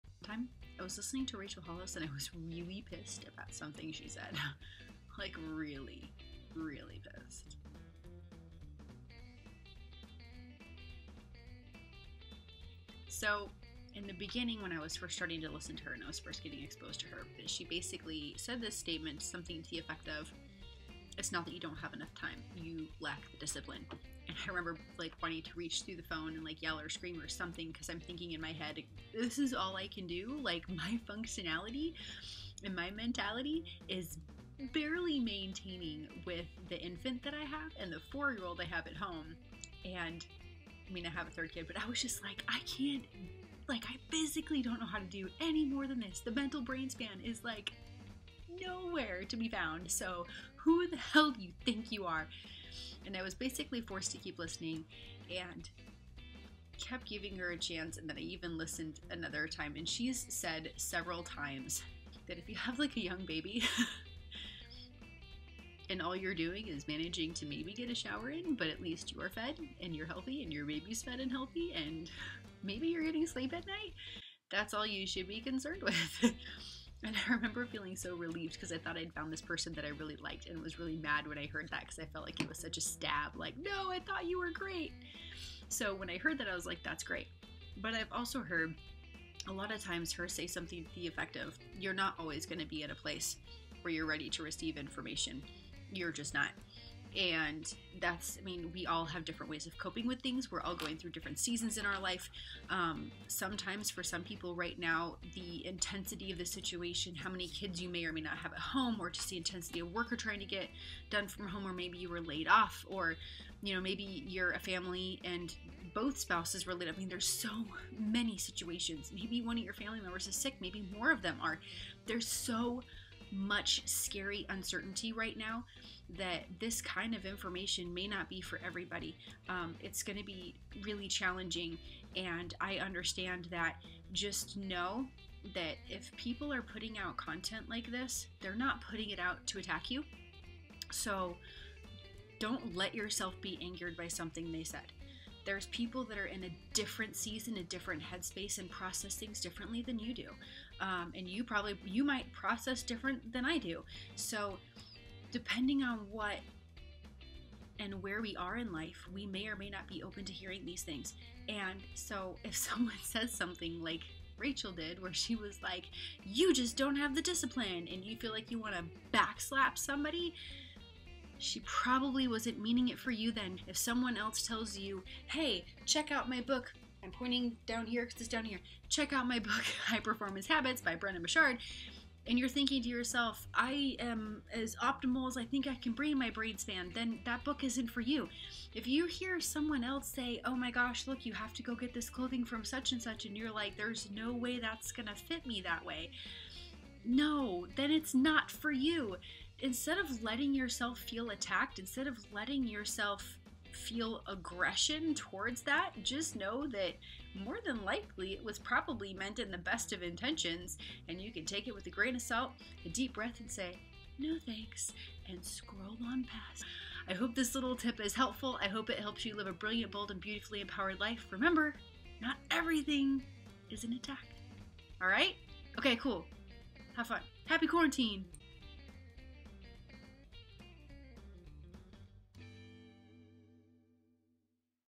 This one time Rachel Hollis pissed me off.
I was truly offended by Rachel Hollis, while I was in the shower one morning.

Let me paint the scene. I'm in my bathroom, trying to shower and feel a little less like a run-down mom. My baby, about 3 months or so, is laying in a rock' n 'play in the bathroom so that I can keep an eye on her. My son, 4 years old at the time, is sitting on my bed playing, my bathroom door open, and I am listening to her. I had only been following her for about 6 months and I wasn't sure I was sold yet, but I knew I liked her style. That was UNTIL I heard her claim that people who claimed they didn't have enough time in a day were not disciplined. They get the same number of hours everyone else does and yet... I about lost my Shhh.